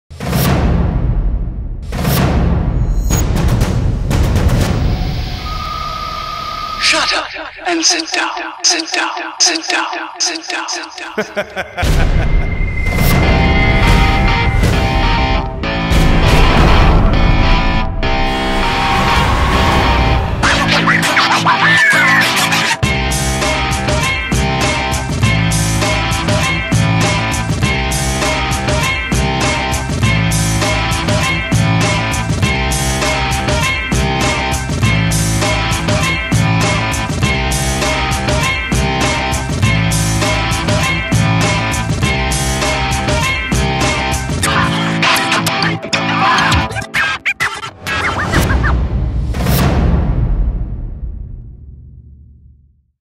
STUDENTS' FILMS SHOWREEL | BROKEN BOX FILMZ | FILM WORKSHOP | ADITI THAKOR
A showreel of work created by the students of Filmmaking workshop by Aditi Thakor under Broken Box Filmz.